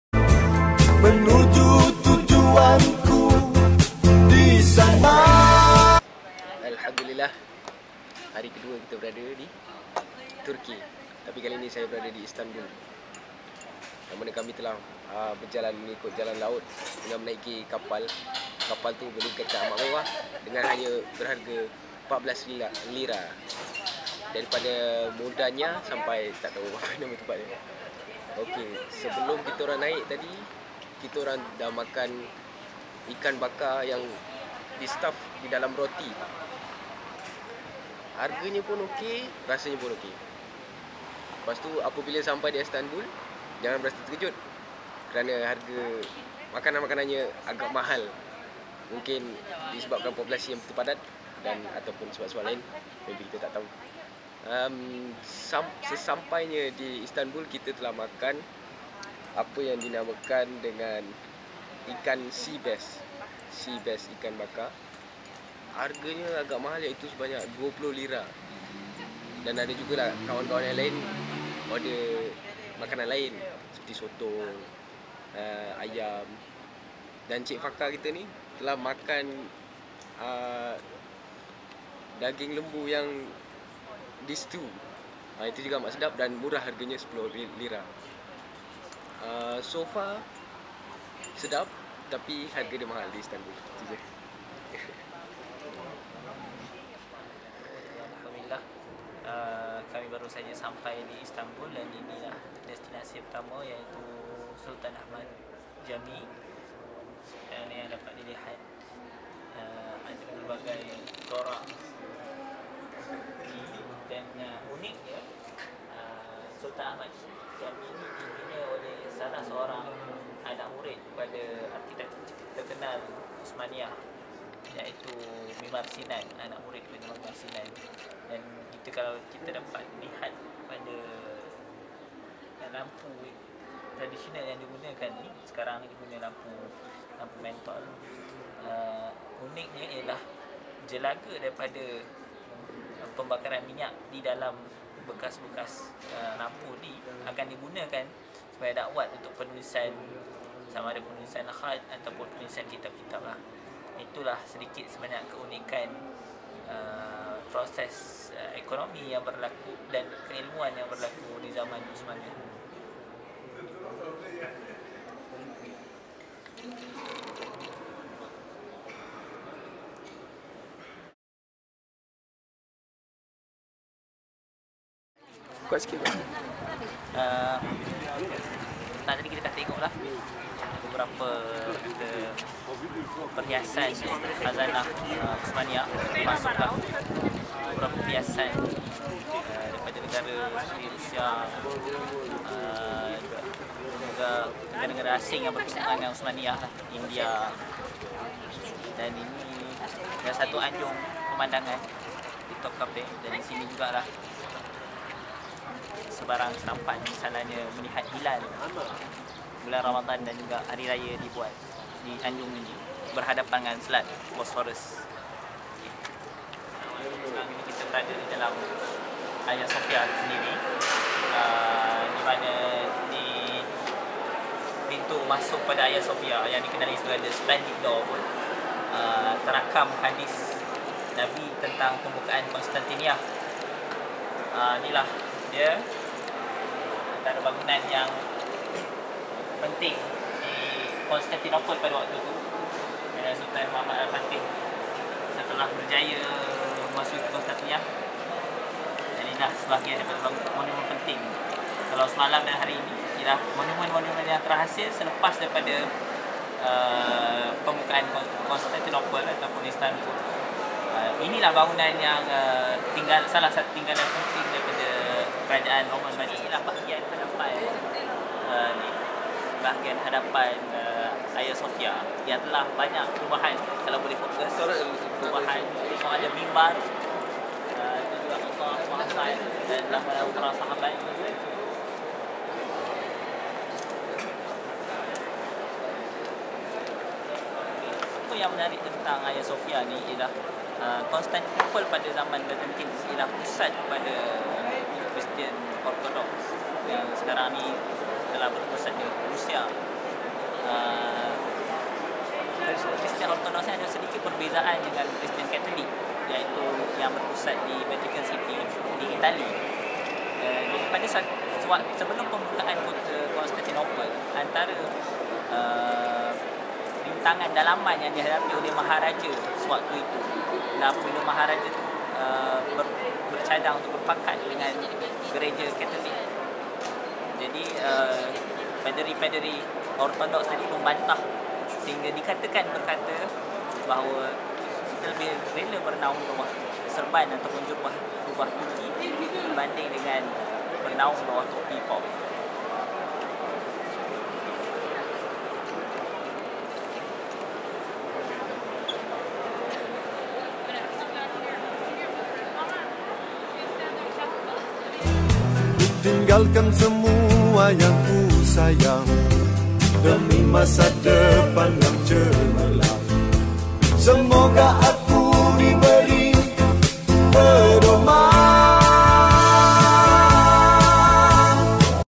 Travelog Istanbul
Bingkisan kembara krew ke Istanbul, ke tempat-tempat bersejarah seperti Masjid Sultanahmet, Hagia Sophia (disebut Ayasofya), istana Topkapi dan istana Dolmahbace.

Uniknya semua tempat bersejarah ini berlatar belakang selat Bosphorus, menunjukkan Istanbul sebagai titik strategik antara benua Asia dan Eropah.

Kami di mengesyorkan beberapa buku sebelum anda ke Turki

1) Secangkir Teh Pengubat Letih, Ustaz Hasrizal Abdul Jamil (budaya dan sikap orang Turki).

2) Quran and Cricket, Dr. Farish A. Noor (ada 1 bab menyentuh kunjungan beliau ke Istanbul).

3) Kuasa Kepemimpinan Al-Fateh, Ustaz Syaari Abdul Rahman (kunci kejayaan Al-Fateh membuka Istanbul).